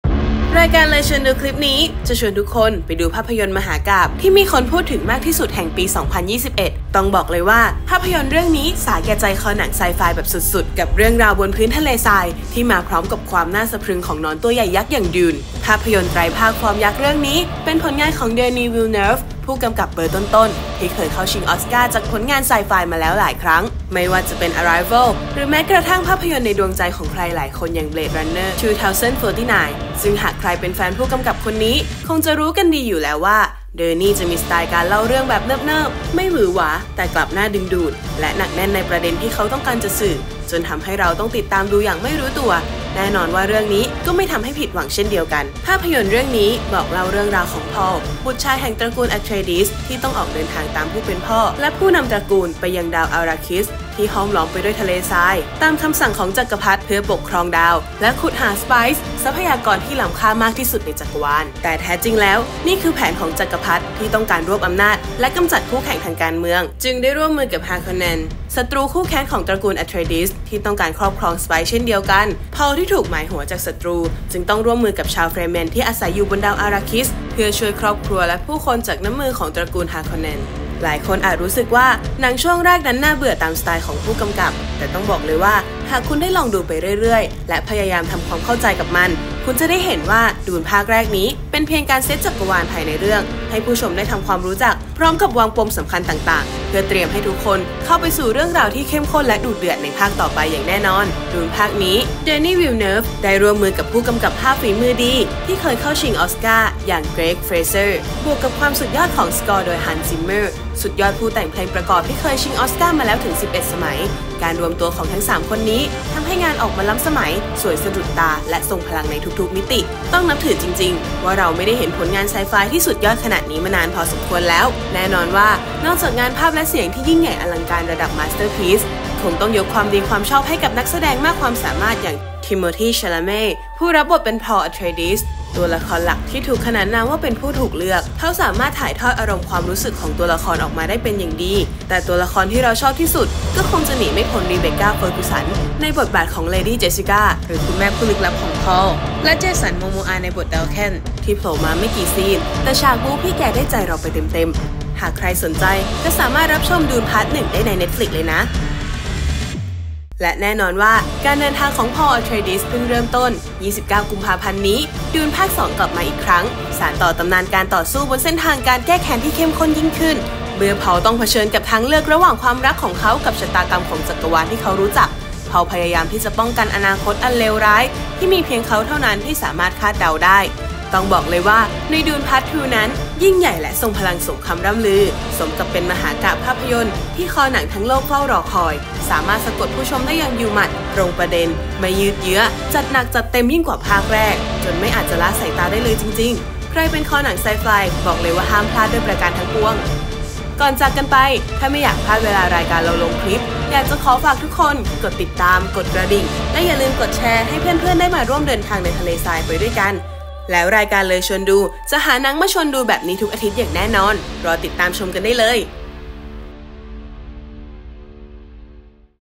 เลยชวนดู : สมรภูมิเจ้าจักรวาล (Dune)
สัปดาห์นี้ เลยชวนดูขอพาทุกคนไปพบกับมหากาพย์ภาพยนตร์ Sci-Fi ที่ถูกพูดถึงมากที่สุดแห่งปี 2021 กับเรื่องราวของ พอล บุตรชายของตระกูลอะเทรดีส ที่ต้องมุ่งหน้าไปยังดาวเคราะห์อาร์ราคิสเพื่อปฏิบัติภารกิจสุดอันตราย โดยมีอนาคตของครอบครัวและจักรวาลเป็นเดิมพัน